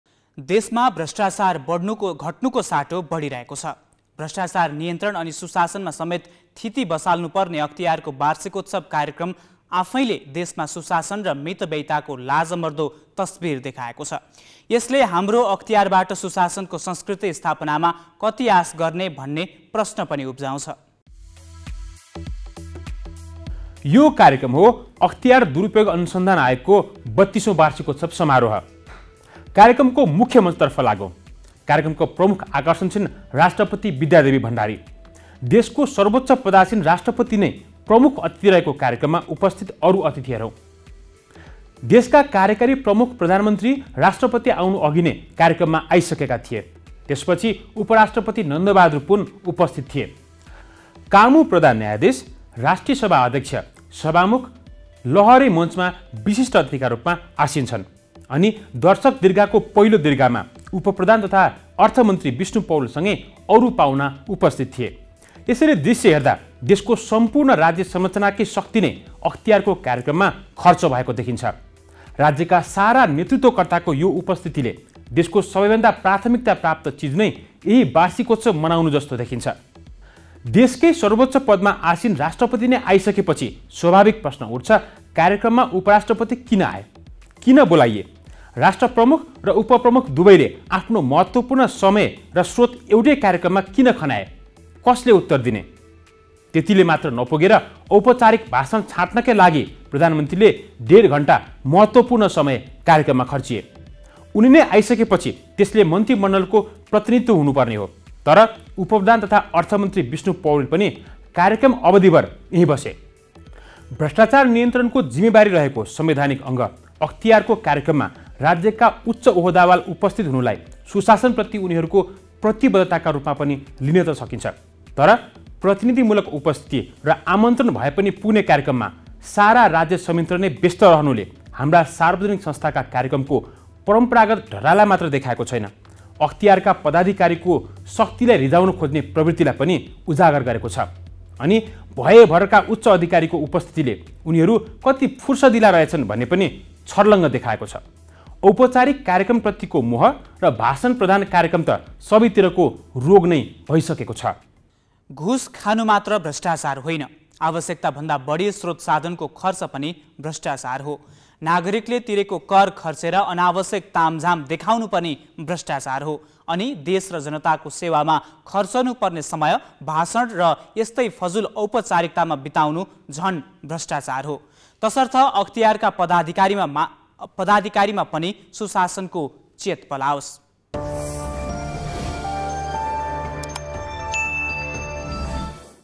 अख्तियारकै कार्यक्रममा मितव्ययिताको खिल्ली | Galaxy News Report
देशमा भ्रष्टाचार घट्नुको साटो बढिरहेको छ । भ्रष्टाचार नियन्त्रण अनि सुशासनमा समेत थिति बसाल्नुपर्ने अख्तियारको वार्षिकोत्सव कार्यक्रम आफैंले देशमा सुशासन र मीतव्ययिताको लाजमर्दो तस्बिर देखाएको छ । यसले हाम्रो अख्तियारबाट सुशासनको संस्कृति स्थापनामा कति आस गर्ने भन्ने प्रश्न पनि उब्जाउँछ ।